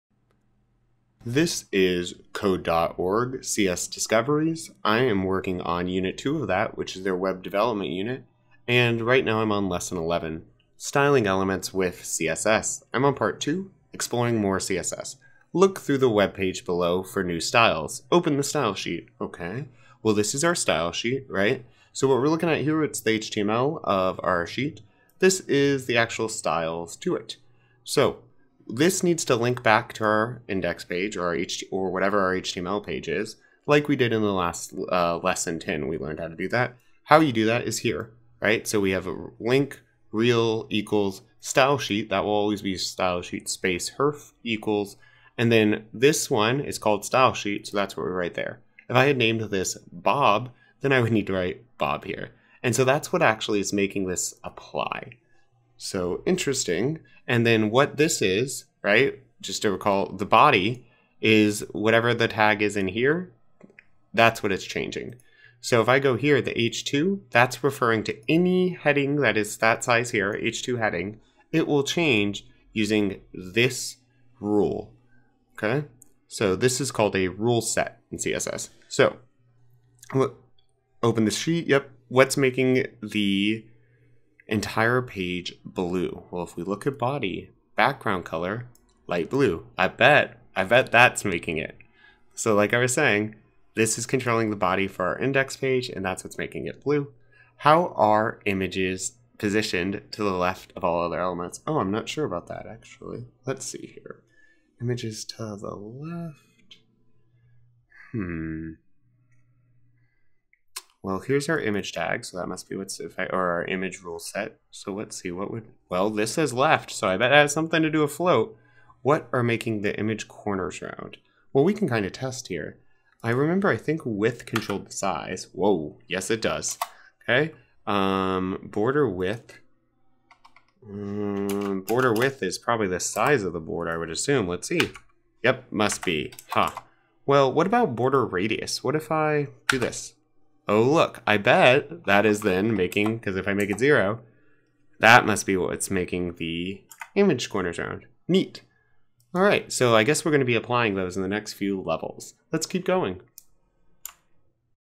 Styling Elements with CSS - Lesson 11.2 - Web Development Code.org CS Discoveries
Styling Elements with CSS is part of the Web Development unit from Code.org's CS Discoveries course. This video tutorial walks through step-by-step how to correctly complete this section of the lesson. I have other tutorials on all levels from the Web Content and HTML chapter of this course.